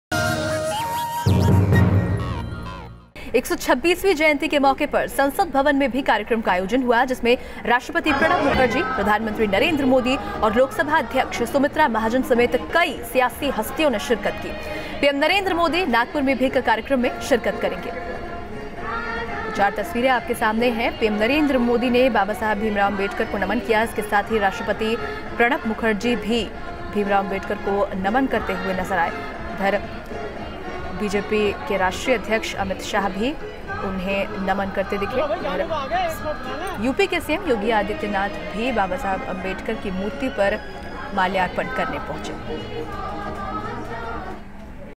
Bhimrao Ambedkar 126th Anniversary: PM Modi Pays Tribute
PM Narendra Modi pays tribute to Dr. Bhimrao Ambedkar on his 126th birth anniversary. Loksabha speaker along with other parliamentarians also paid tribute.